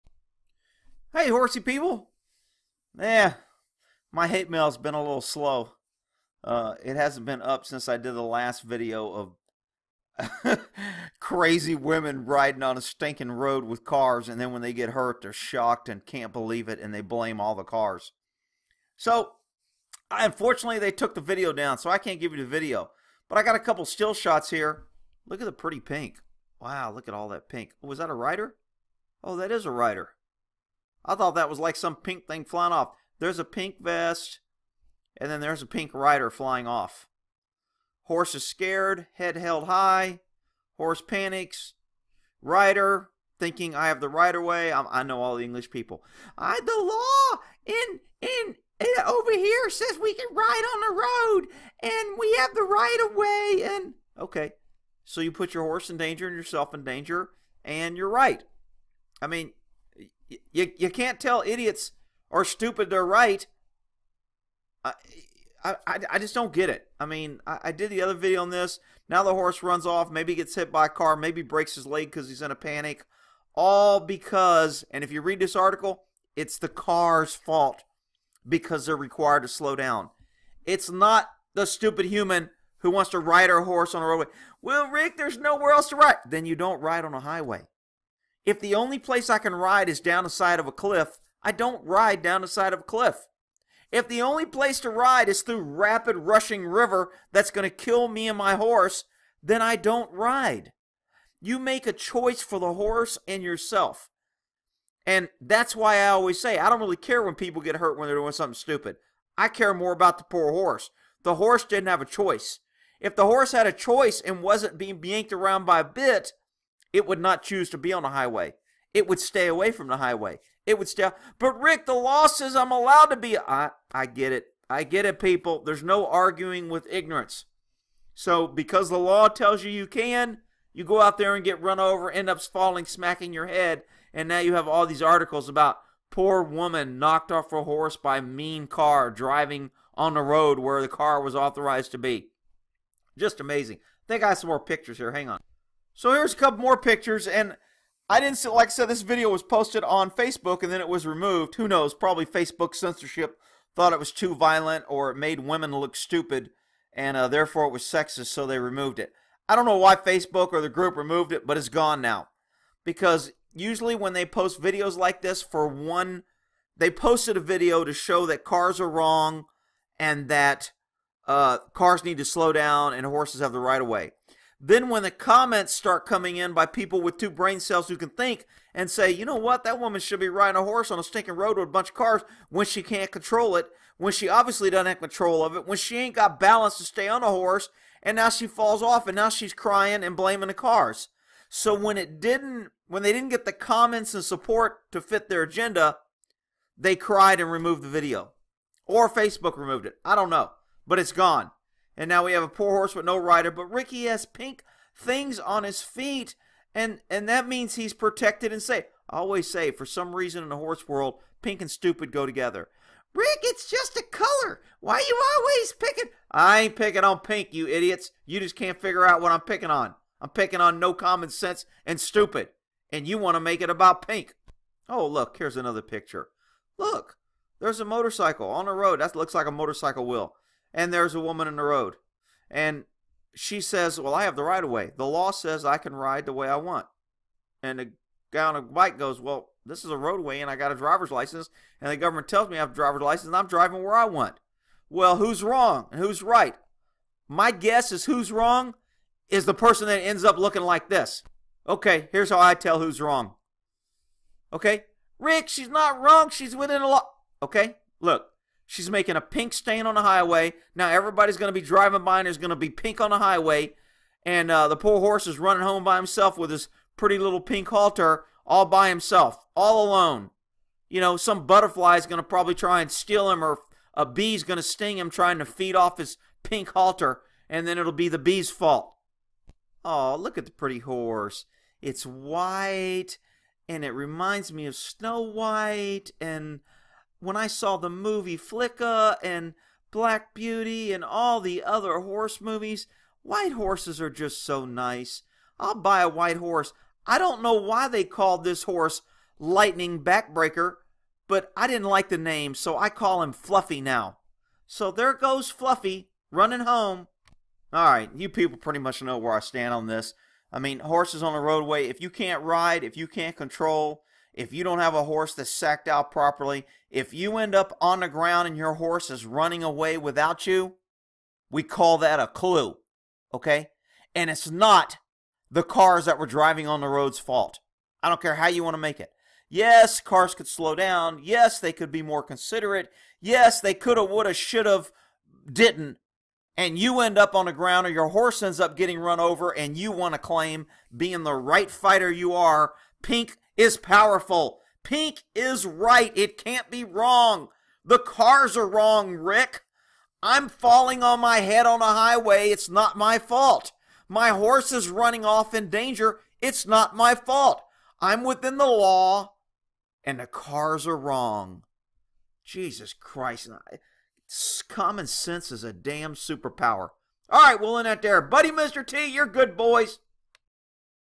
Mean Car That Hates Pink Spooks Horse & Makes Poor Woman Fall Off
thinklikeahorse ItIsNeverTheHorsesFault Here is a Tip: Don't ride a horse that you can't handle on the roadway with cars and just because a horse spooks does not mean if you fall off it is the horse's fault.